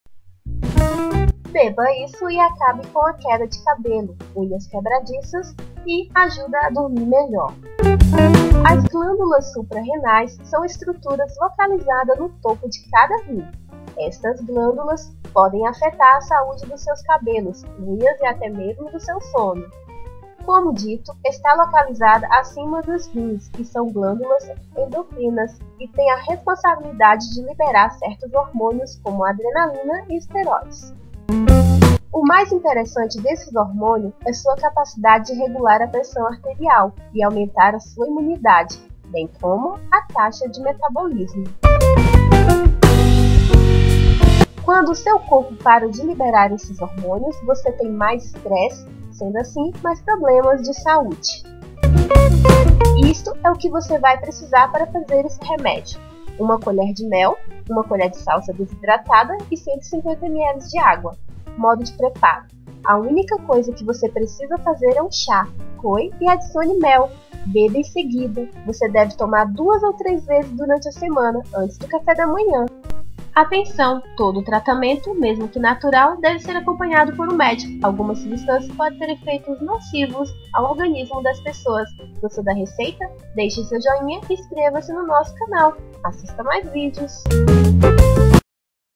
❤ Beba isso e acabe com QUEDA de CABELO, UNHAS QUEBRADIÇAS E ajuda a DORMIR MELHOR!
As glândulas supra-renais são estruturas localizadas no topo de cada rim. Estas glândulas podem afetar a saúde dos seus cabelos, unhas e até mesmo no seu sono.
Como dito esta localizado acima dos rins e são glândulas endócrinas. E tem a responsabilidade de liberar certos hormônios, como adrenalina e esteróides.
O mais interessante desses hormônios,é sua capacidade de regular a pressão arterial e aumentar a sua imunidade, bem como a taxa de metabolismo.
Quando o seu corpo para de liberar esses hormônios, você tem mais estresse, e problemas de saúde.

ATENÇÃO!
Todo tratamento, mesmo que natural, deve ser acompanhado por um médico.Algumas substâncias podem ter efeitos  nocivos ao organismo das pessoas.